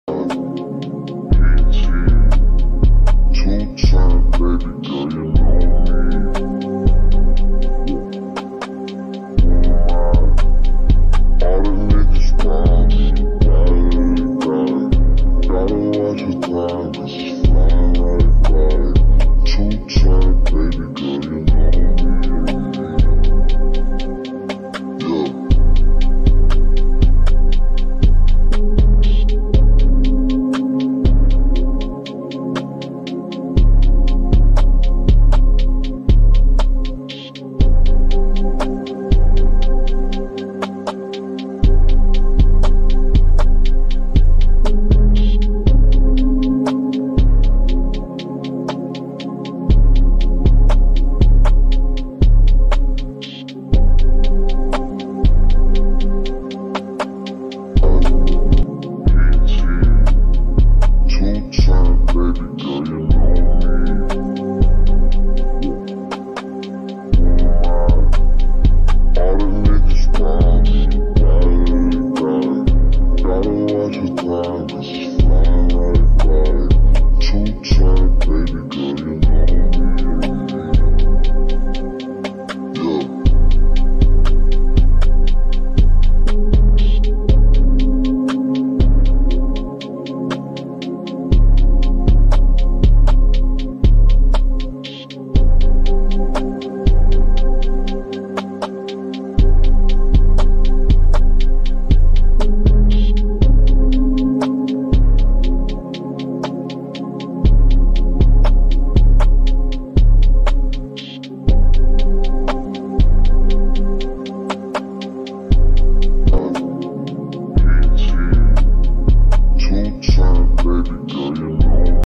Would You Rather...? Bike Showdown
Grab your helmet, hop on your current bike, and embark on this thrilling Would You Rather  bike comparison journey with us. A fun this or that bike edition!!